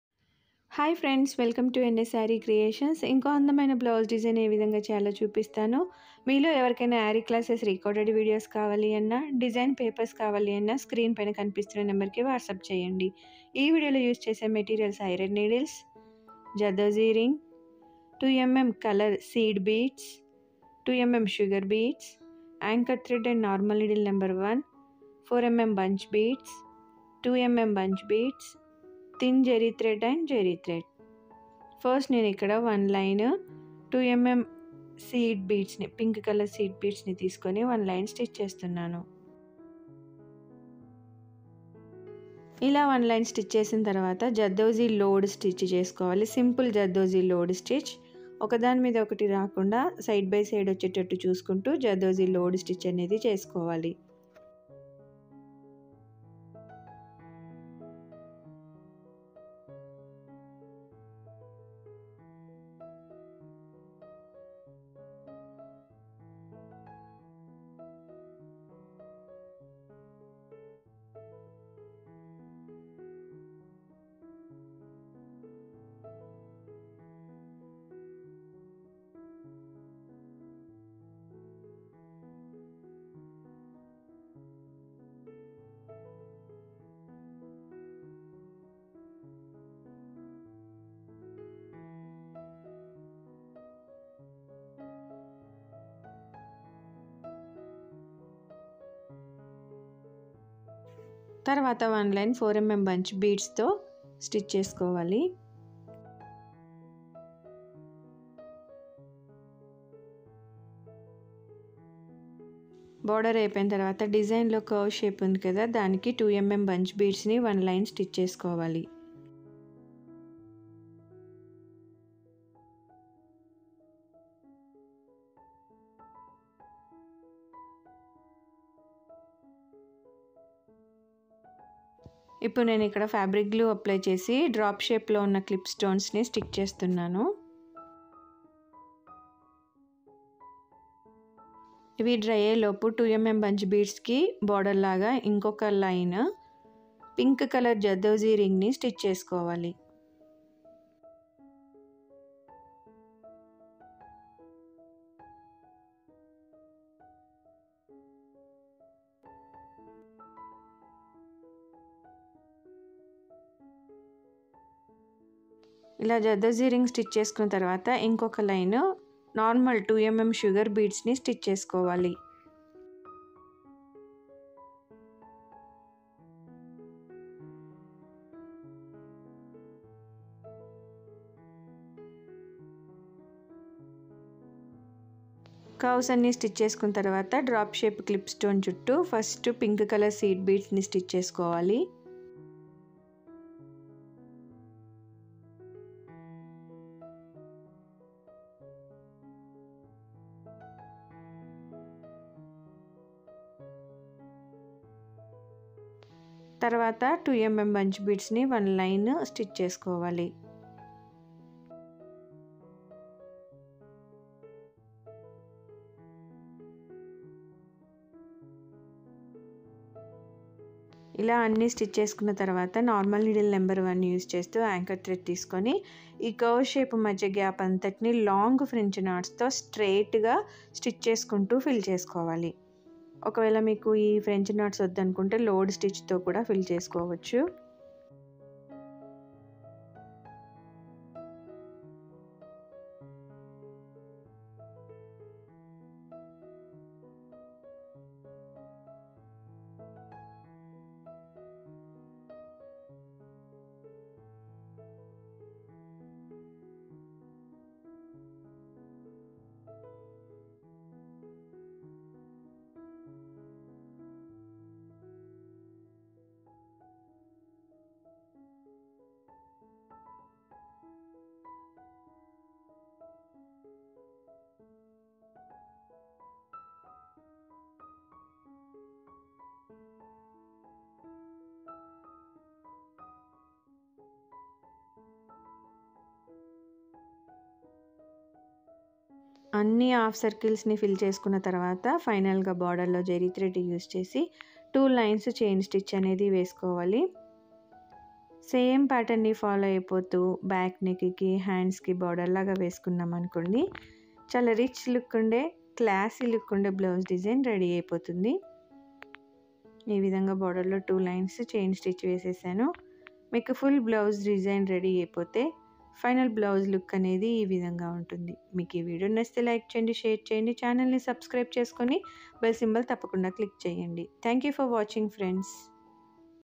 Trendy Blouse design with Aari Work | Maggam works
Used Materials in this Video:
Aari Iron Needles
Normal needle no :1
2mm sugar  beads
2mm seed beads
4mm &2mm bunch beads
drop clip stones
Anchor Thread
Zari thread
Thin Zari Thread